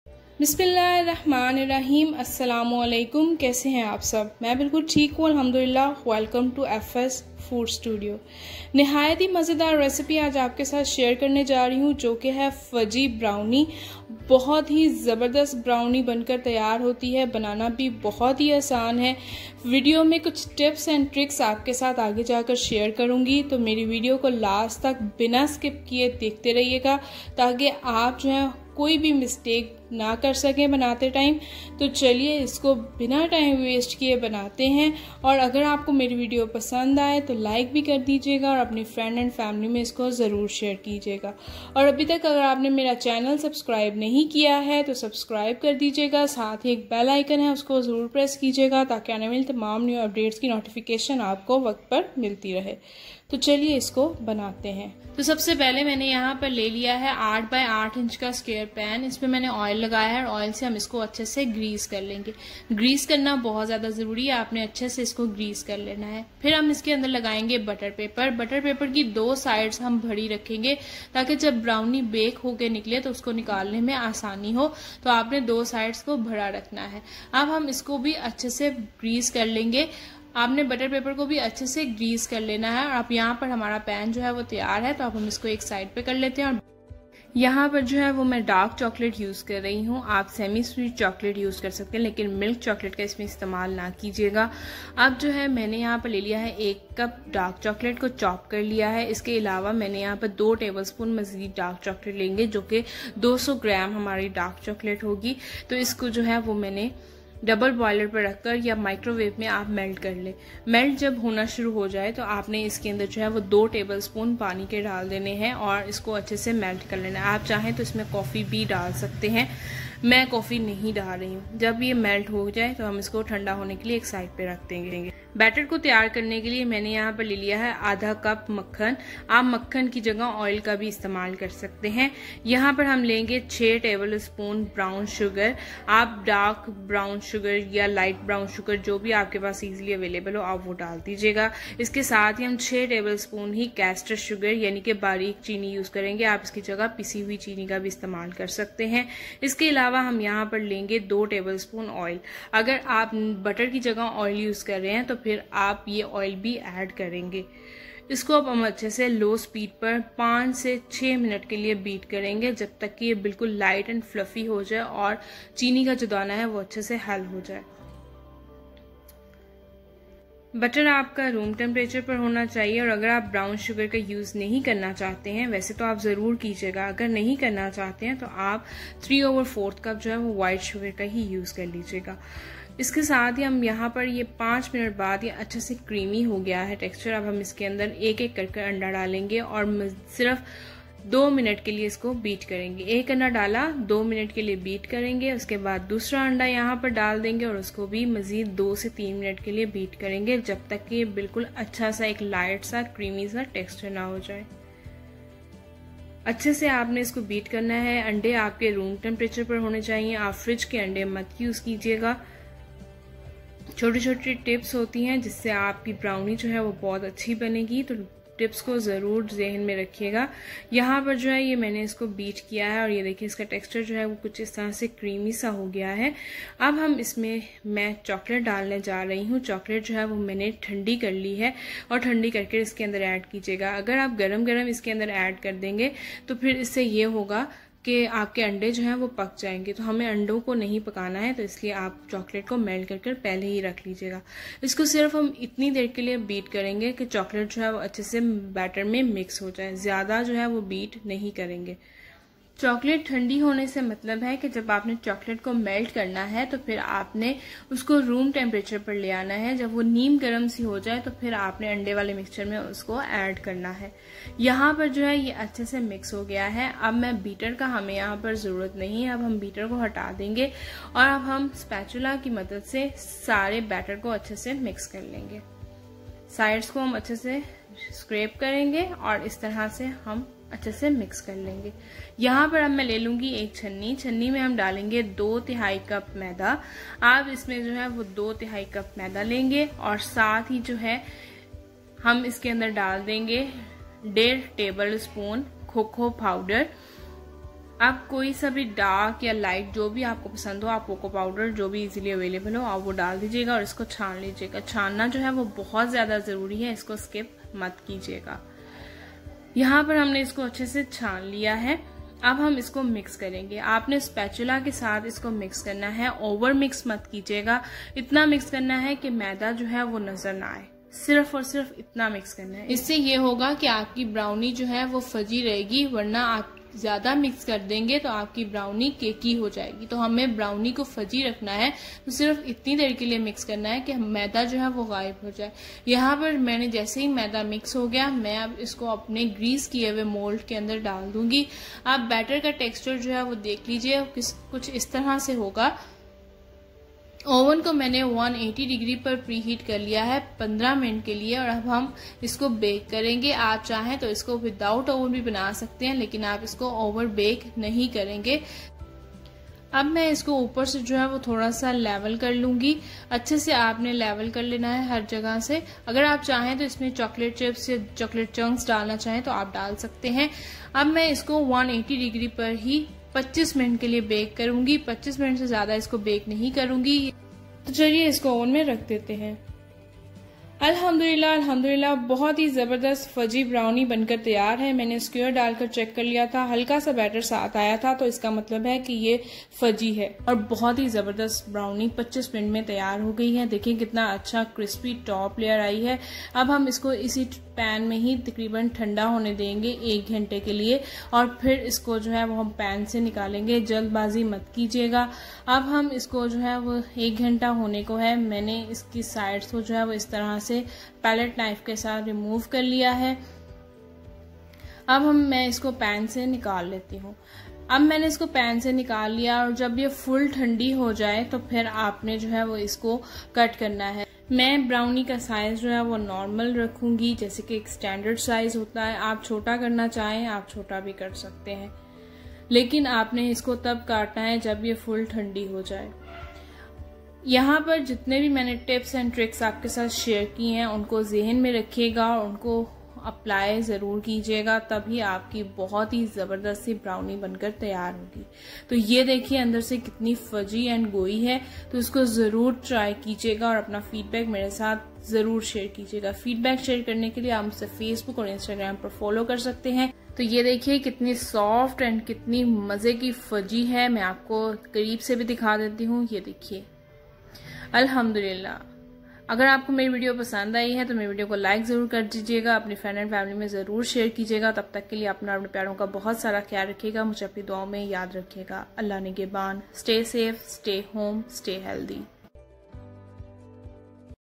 Chocolaty & Gooey FUDGY BROWNIES Recipe || Detailed Recipe with many Tips || Fs Food Studio
World best Fudgiest Brownies in my Best Brownies Recipe , perfect crisp , Crackly top , Super Fudgy Centre , Chewy and Gooey In all the Right Place , studded with Melted Chunks of Chocolate
Easy to follow recipe video with ful demonstration
A must try Recipe! ENJOY 🙂

Feel free to go through my other recipes, many more to Come Inn Sha Allah
 Link of my YouTube channel👇

For any queries or Feedback ,